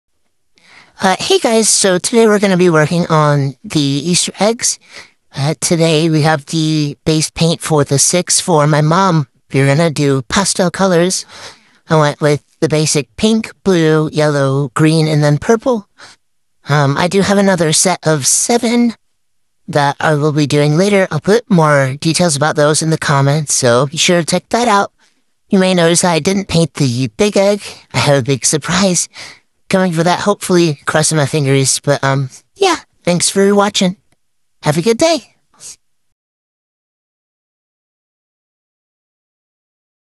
The eggs for 2024!
Part 1 of the eggs for 2024! These will be the first set, and more traditional style than the others. Be sure to comment some ideas and follow to see how they turn out!

voice changer - trickster on capcut (sorry if the audio is weird - I've never done something like this before lol)